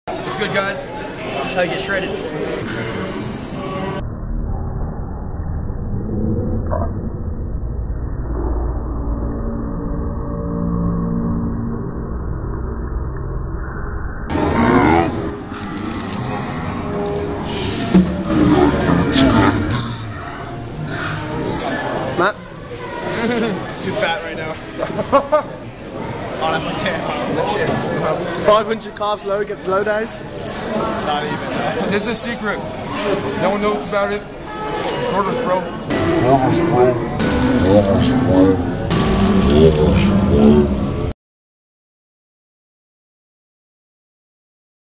Six Packs Abs and Fast Food.. Wtf?!
Eating some burger king with Matt Ogus y company in the UK.